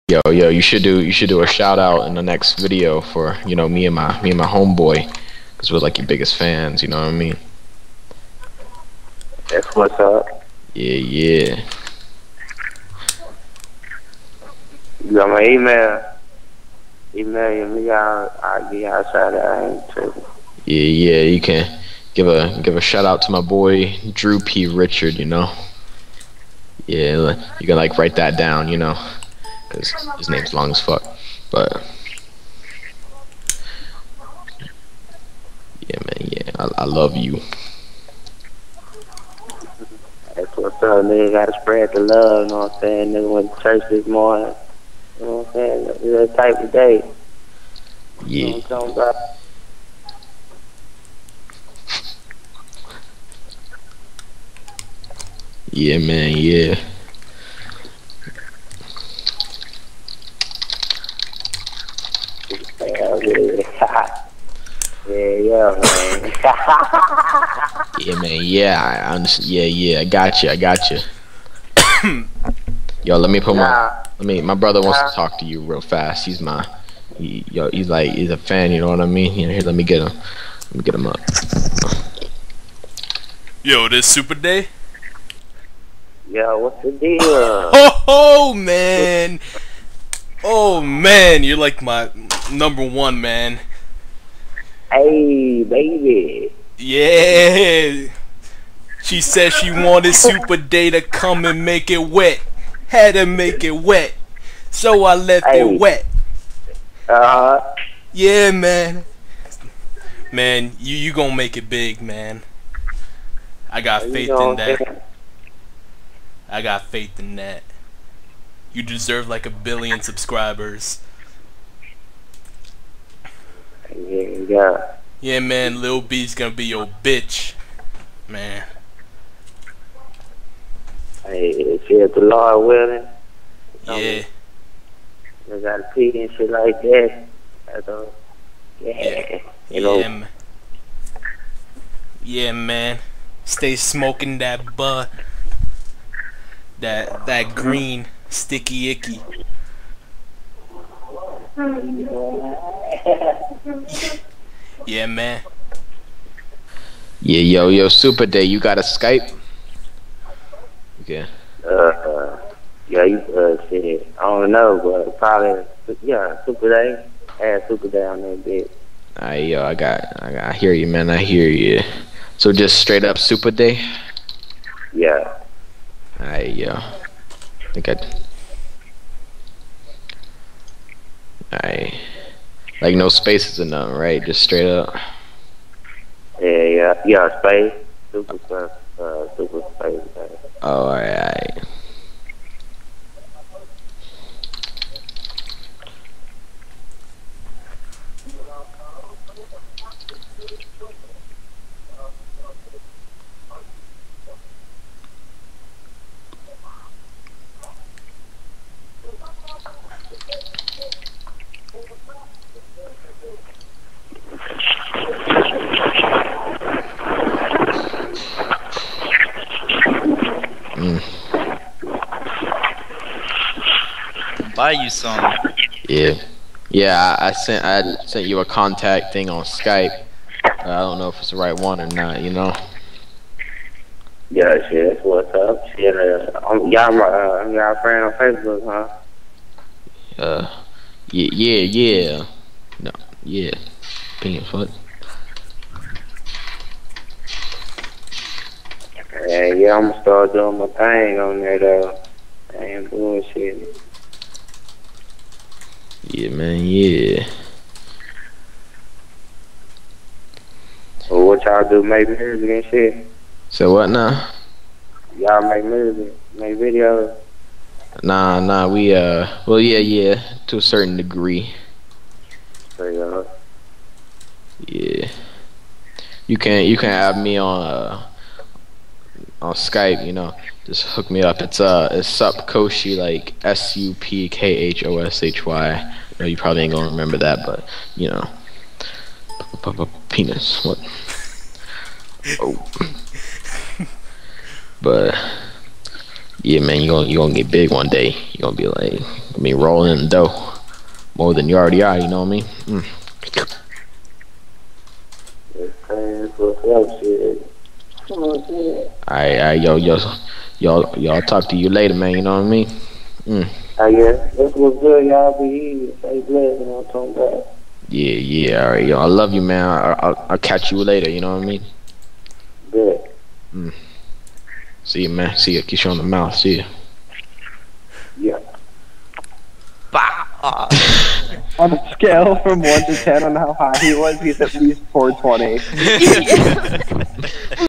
Skype Call with Supa Day
Me and the boys called up Supa Day to say what's up. You know.